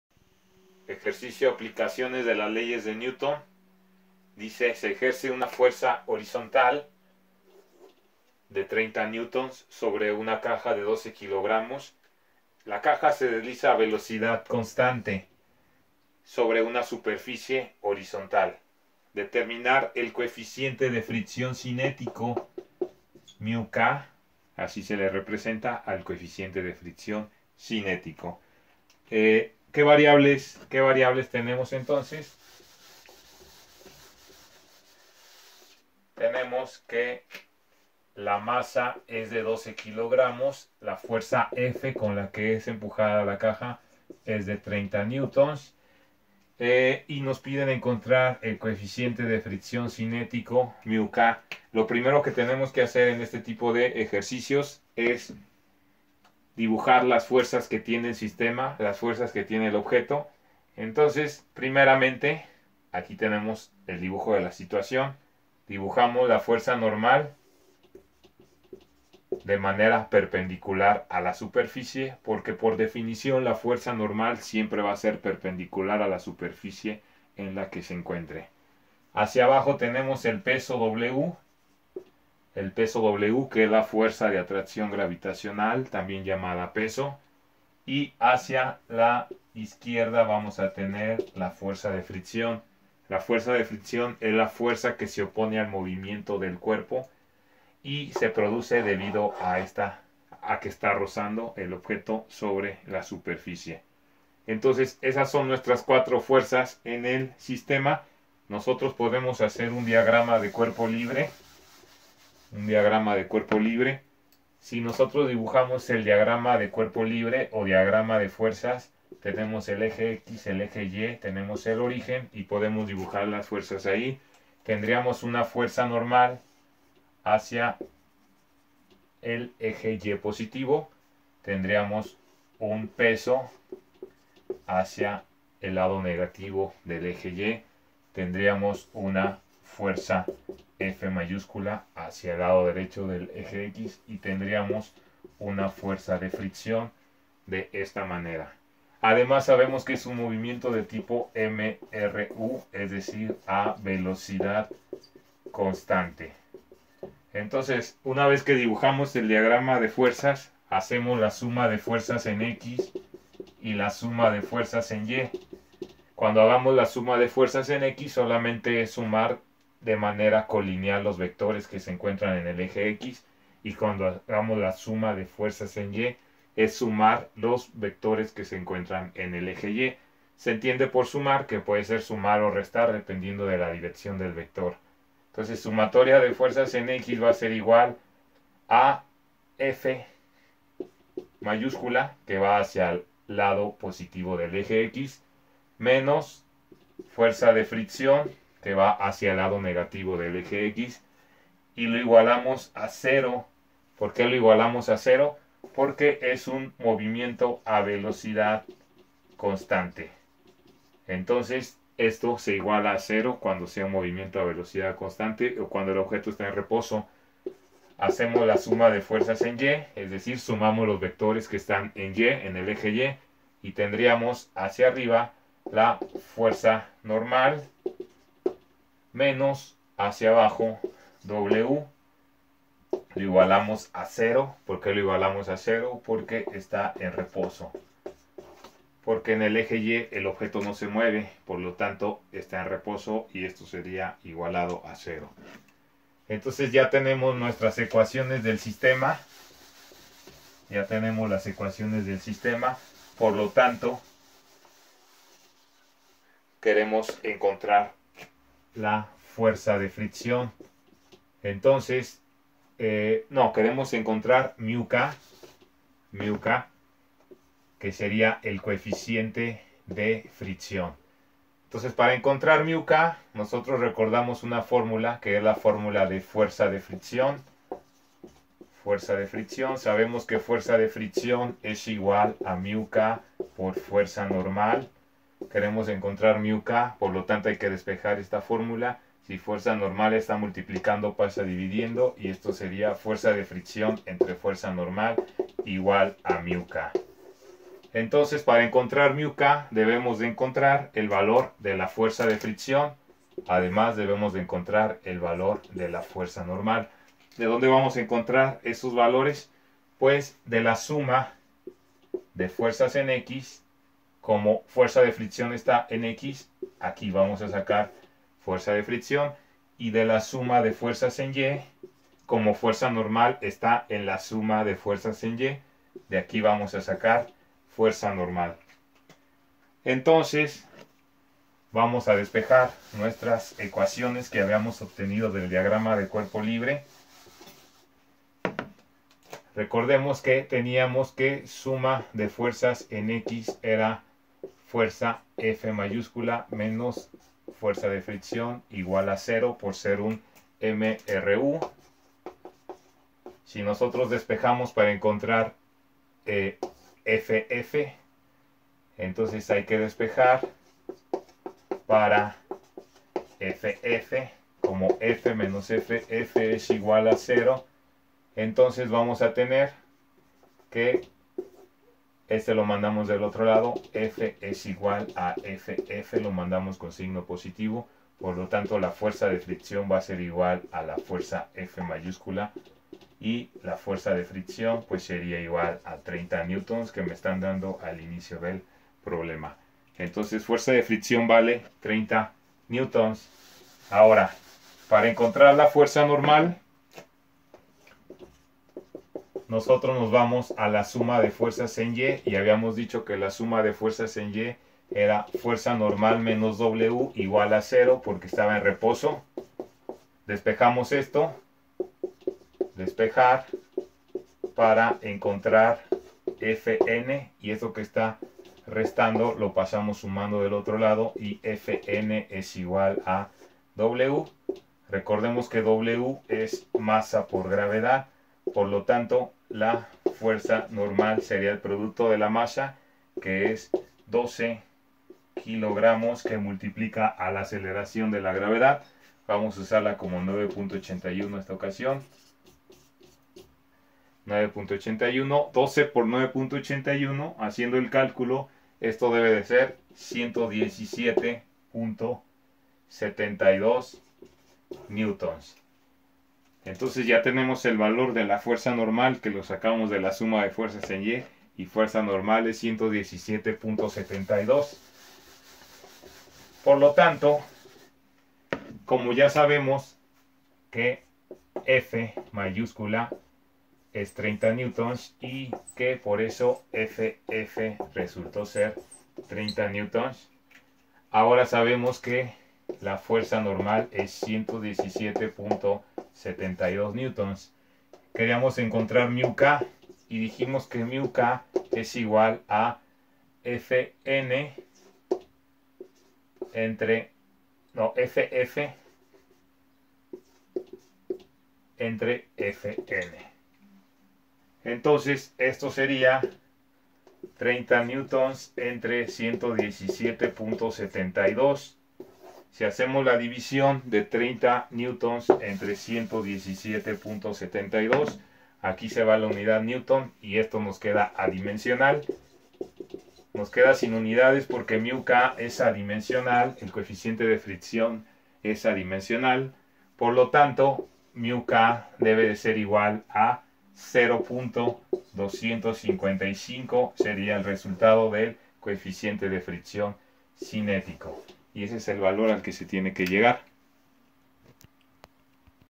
APLICACION LEYES DE NEWTON SUPERFICIE HORIZONTAL EN MRU
Se resuelve ejercicio de Aplicación de las Leyes de Newton. El ejercicio dice: Se ejerce una fuerza horizontal de 30 N sobre una caja de 12 kg que se desliza a velocidad contante (MRU) sobre una superficie horizontal. Determinar el coeficiente de fricción cinético.